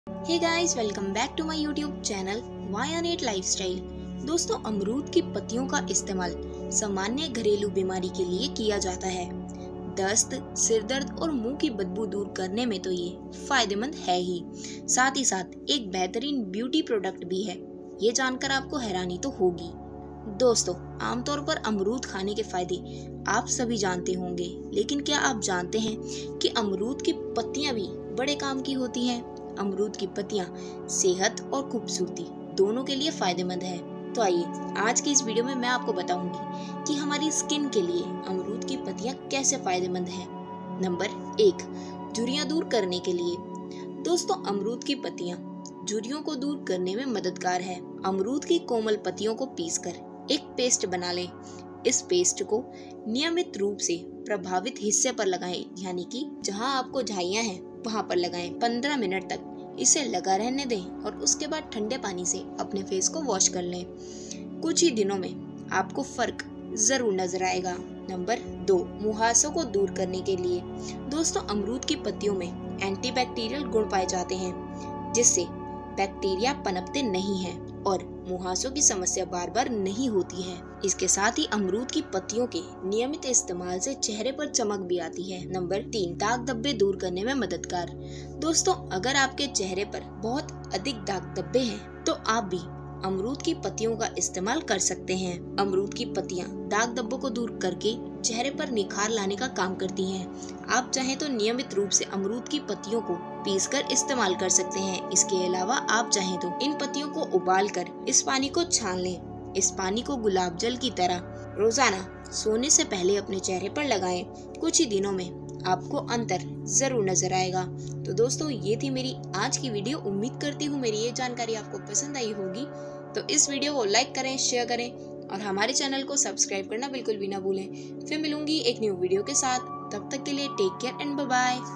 Guava Leaves Face Pack: अमरूद के पत्तों से निखारें अपनी त्वचा | Guava Benefits For Skin
Amrood Ki Patti Ke Fayde For Skin, Guava Leaves Benefits, Guava Leaf Benefits For Skin

👉 Press bell icon 🔔 also for Notifications

* चमकती दमकती स्कीन पाने के लिए आसान Diet Plan

* पीरियड्स की परेशानी को कैसे दूर करें

* चेहरे के लिए पान के पत्ते के फायदे